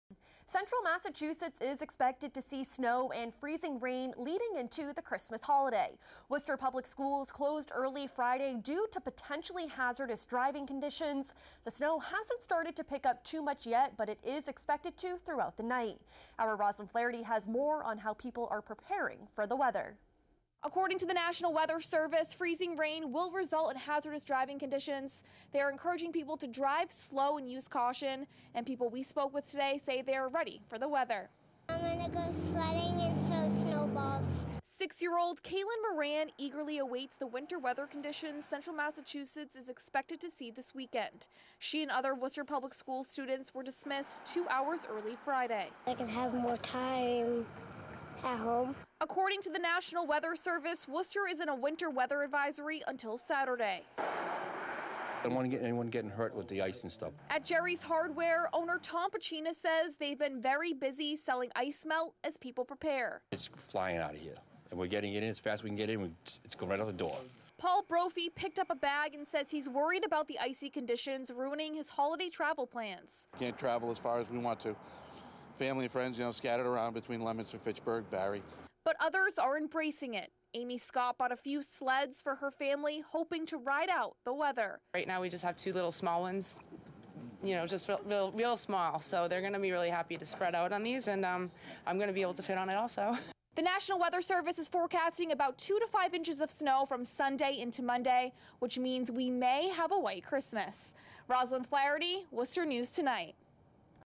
WNT Snow Preps - December 22nd, 2017
Roslyn Flaherty reports- A wintery mix in the forecast forces Worcester Public School to close early as residents prepare for the storm.